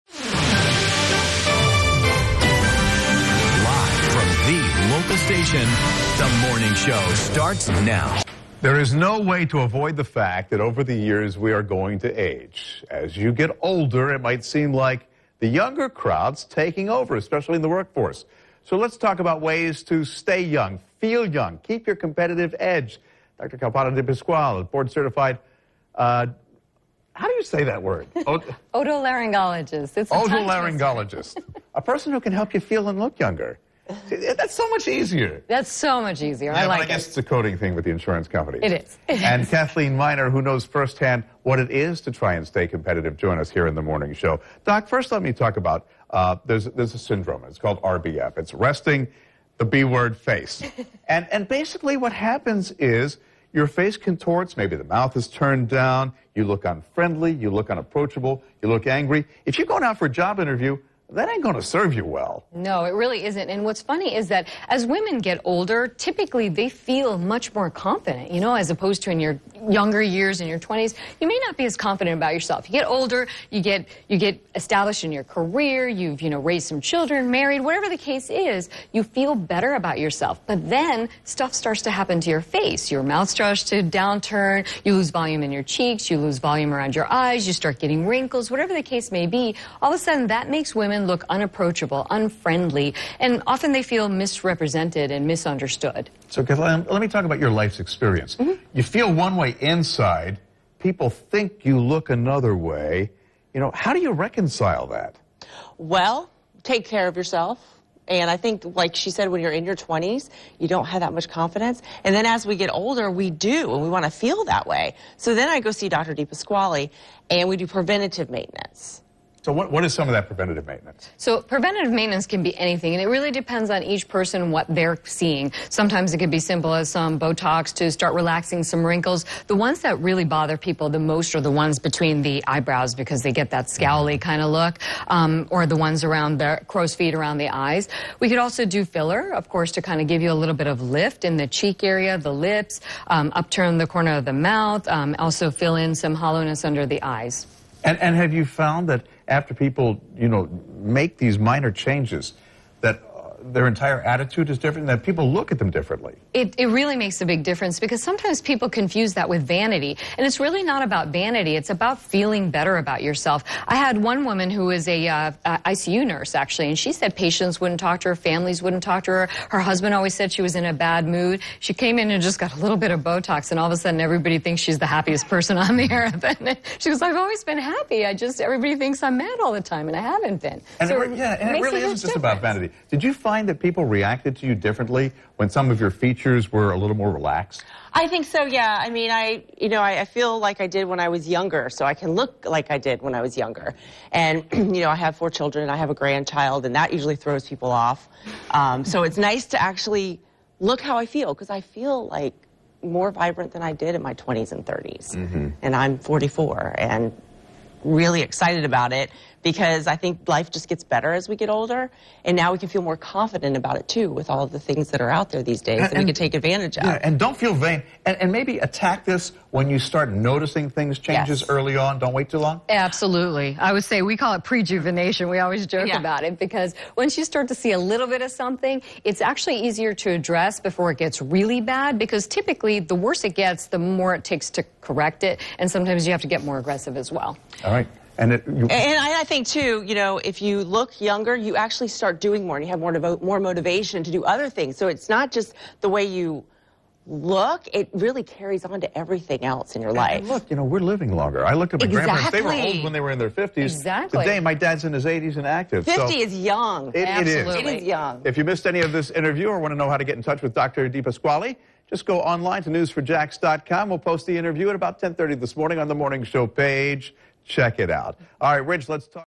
How To Keep A Competitive Edge As You Age. WJXT 12/05/16
There is no way to avoid the fact that over the years we are going to age. As you get older, you may feel that the younger crowd is taking over! How can you stay competitive in the work force? Check out our TV appearance on WJXT from December 2016 where Dr. D explains how keeping a rejuvenated appearance may help.